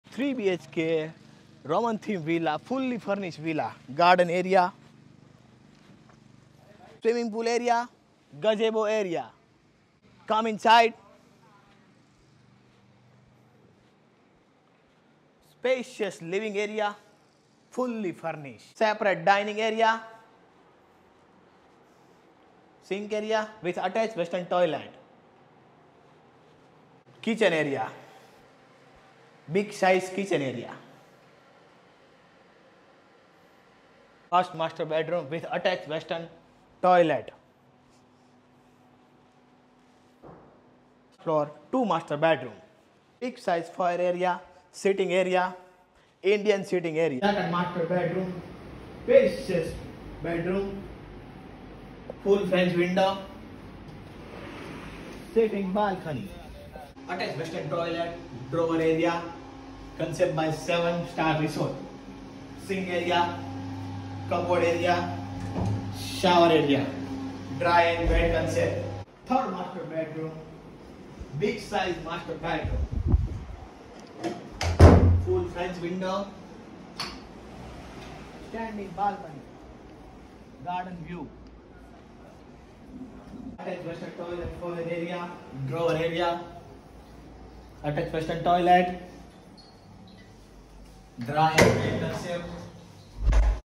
3BHK યુરોપીયન મેન્શનથીમ વિકેન્ડ વિલાવેસ્માચોકડી | પલસાણા નવસારી મુંબઈ હાઈવે NH-8 રોડ | નવસારી સુરત.
3BHK યુરોપીયન મેન્શન થીમ વિકેન્ડ વિલા | 4 સાઈડ ઓપન વિલા | પર્સનલ સ્વિમિંગપુલ | પર્સનલ ગાર્ડન | વેસ્માચોકડી | પલસાણા નવસારી મુંબઈ હાઈવે NH-8 રોડ | નવસારી સુરત |